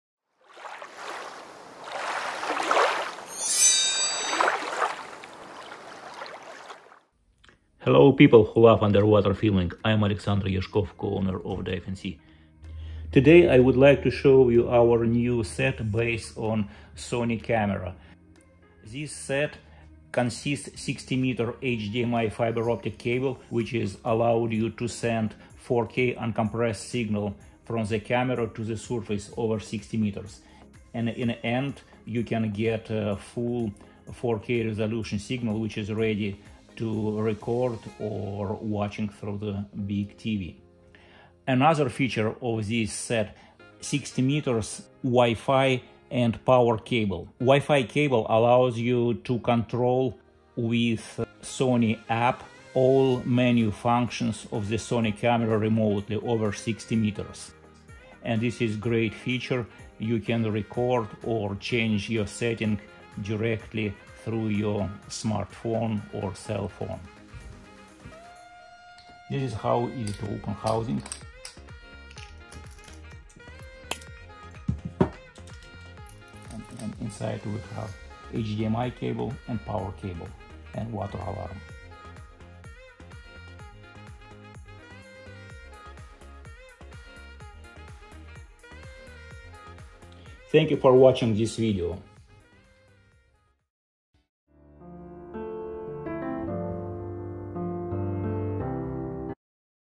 Wired 60m Remote control Wi-Fi Underwater System, 4K HDMI 60m Live View
Underwater Wi-Fi set DNC-S2 Sonic based on Sony RX0 II camera allowed you to control remotely underwater filming over 60 meters.
Camera powered from the surface can use 24 hours non-stop.
HDMI 60m cable sends uncompressed 4K resolution Live View stream to the surface.
WiFi wired 60m cable allowed you fully controlled all settings of the Sony camera in real time.